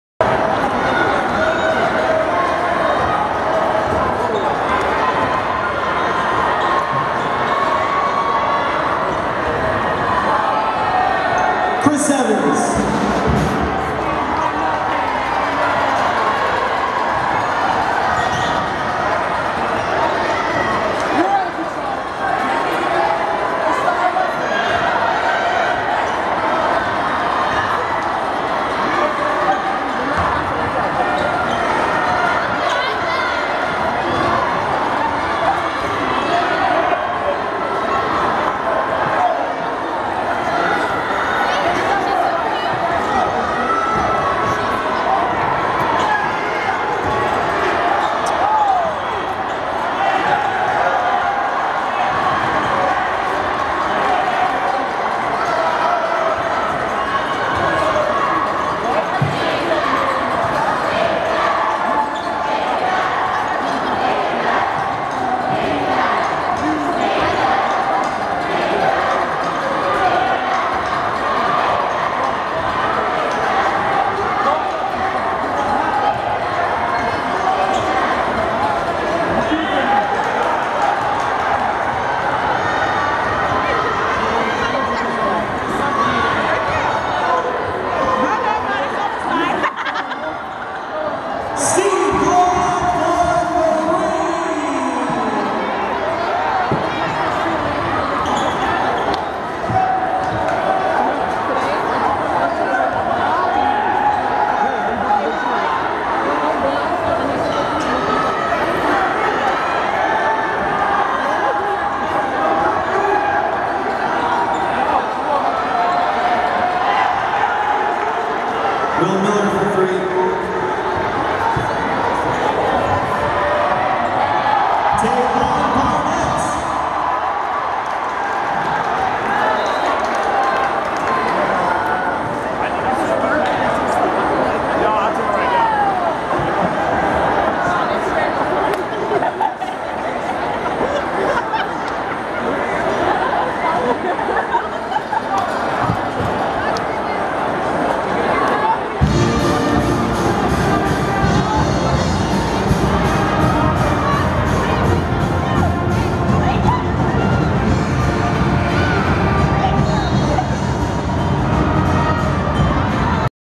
Mt St Mary's vs Sacred Heart - Men's Basketball - 2nd Half - Barnett Dunk - Injured - Feb 06, 2014
Mount St. Mary's Mountaineers vs Sacred Heart Pioneers
Men's Basketball  -  2nd Half Action
Chris Evans scores the basket off glass
Steve Glowiak deep 3-pointer
Will Miller 3-pointer
De'von Barnett slam dunk  -  injured on the play
William H. Pitt Center  -  Fairfield, Connecticut